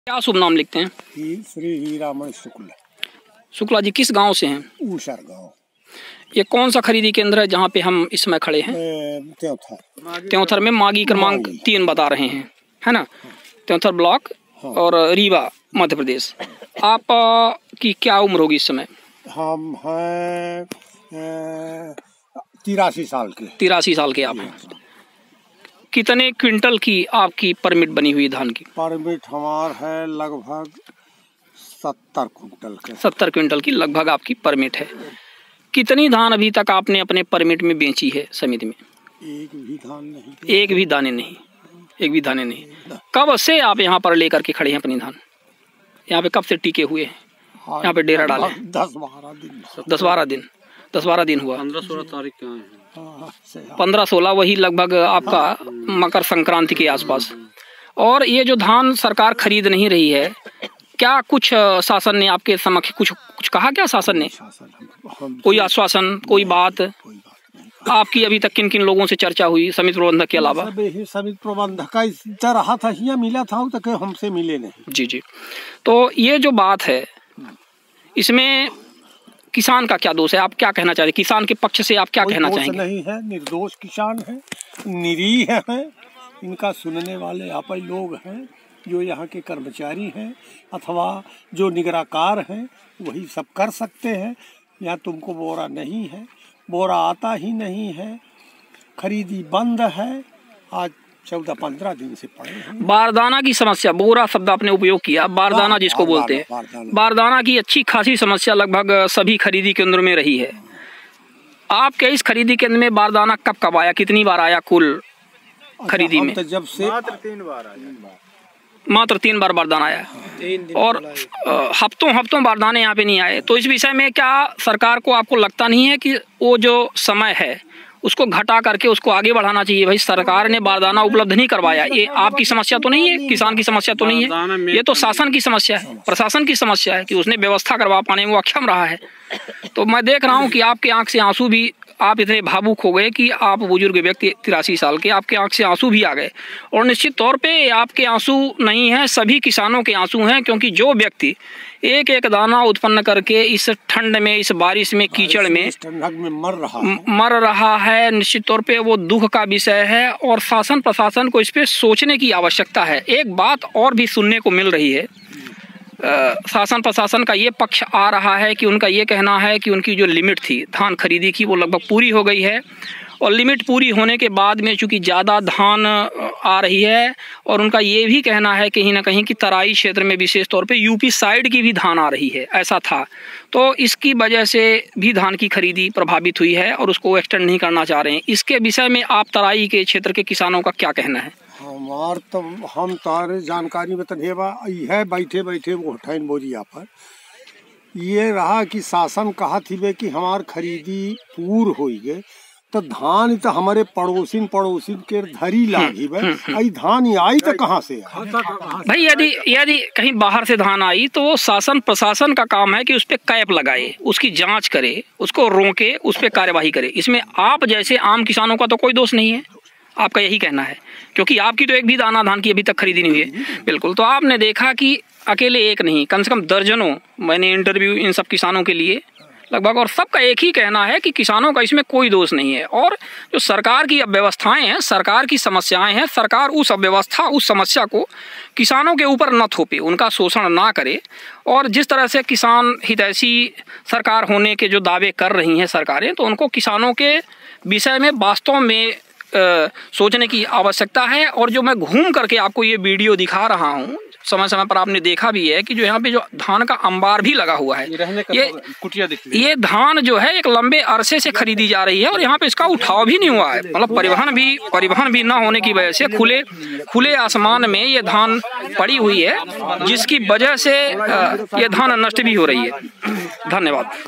83 वर्ष के किसान हीरामणि शुक्ला की आंखों में आ गए आंसू ।। वाह रे किसान हितैसी सरकारें ।। रीवा मप्र
83 वर्ष के किसान हीरामणि शुक्ला की आंखों में आ गए आंसू ।। वाह रे किसान हितैसी सरकारें ।। रीवा मप्र ।। मागी खरीदी केंद्र क्रमांक 3 में किसानों की हालात हुई खस्ता ।। किसान सब काम छोंड़ कर अपनी अपनी धान की कर रहे पहरेदारी ।। शिवानन्द द्विवेदी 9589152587